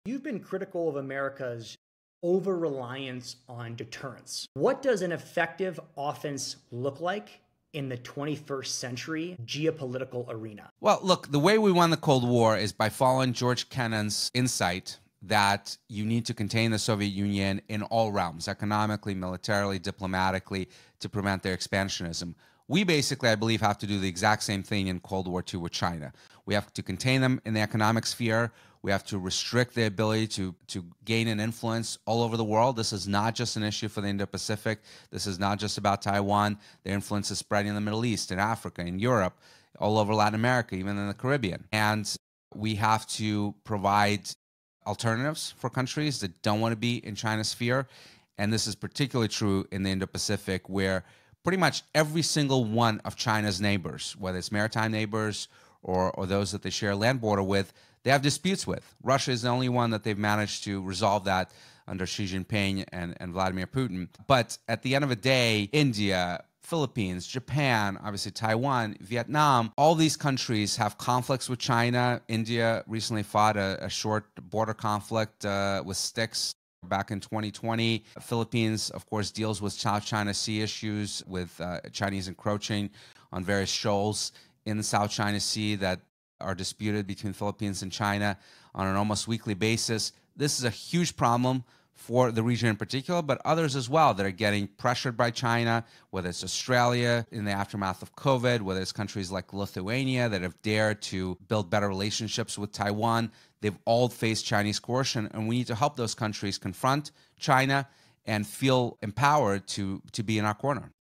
Exploring modern geopolitical offense with CrowdStrike’s Dmitri Alperovitch. Big insights.
Dmitri Alperovitch is a cybersecurity expert, co-founder and former CTO of CrowdStrike, a leading company in threat intelligence and cyber defense. He is the author of World on the Brink: How America Can Beat China in the Race for the 21st Century, which examines global security, technology, and geopolitics. Alperovitch also founded Silverado Policy Accelerator, a think tank advancing U.S. leadership in national security, economic competitiveness, and innovation.